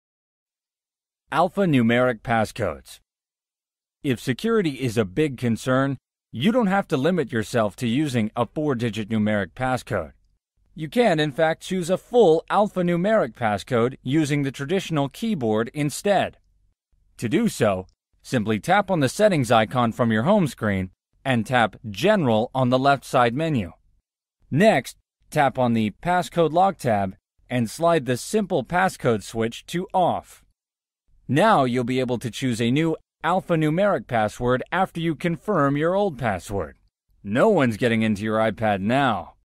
Setting An Alphanumeric Passcode on your iPad
A lesson from The iPad Video Coach that shows you how to set an alphanumeric passcode on your iPad instead of the traditional 4 digit passcode.